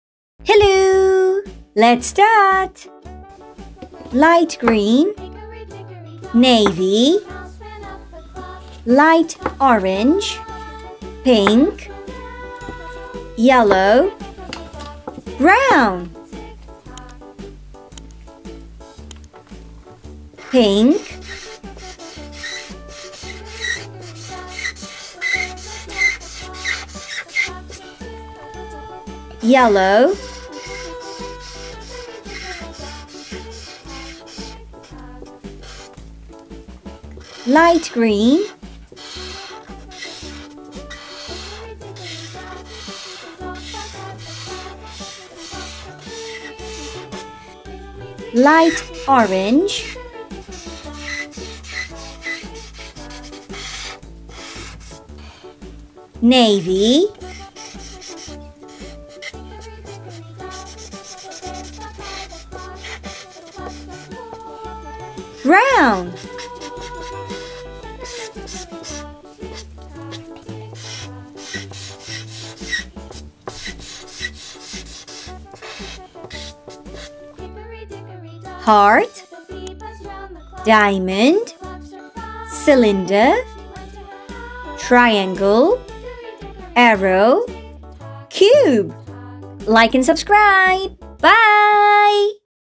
Fun and Educational Shape Drawing for Kids | Learn Shapes Through Art and Play - 138
In this video, we'll be exploring the world of shapes through drawing and creative activities. Children will learn about basic geometric shapes such as circles, squares, triangles, and more, through engaging drawing exercises that will enhance their understanding of shapes in a playful way.

Each shape will be accompanied by a fun drawing activity that reinforces its characteristics, making it an enjoyable and educational experience for young learners. Join us as we embark on a delightful journey of learning and creativity!

Let's make learning shapes an exciting adventure for kids!

- learn shapes
- drawing for children
- educational drawing
- geometric shapes
- kids learning
- easy drawing tutorial
- shape art
- preschool learning
- QooEnglish